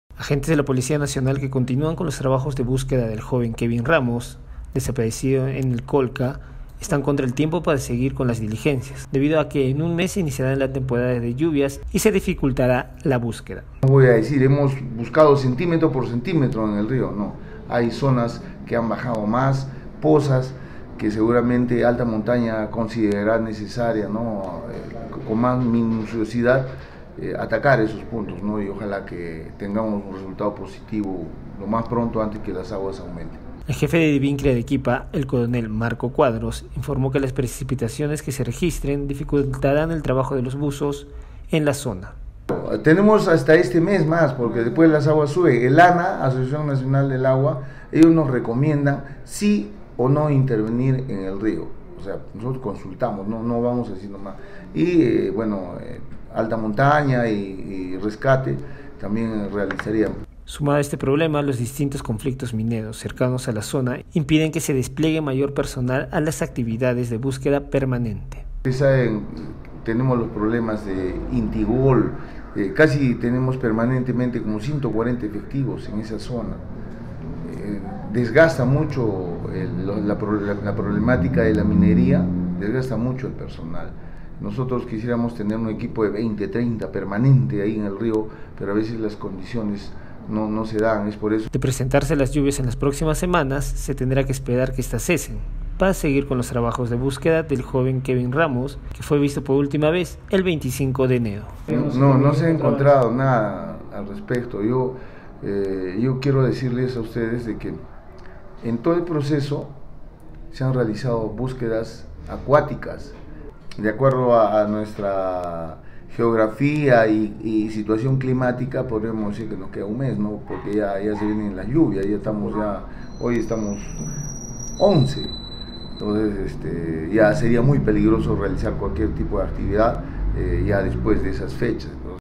Lluvias dificultarán búsqueda de Kevin Ramos
Policías de Divincri Arequipa intensificarán los trabajos de búsqueda del joven Kevin Ramos, quien despareció el 25 de enero en el valle del Colca. Los esfuerzos para encontrar los restos del menor se hacen antes que se inicie la temporada de lluvias, lo que perjudicaría las diligencias.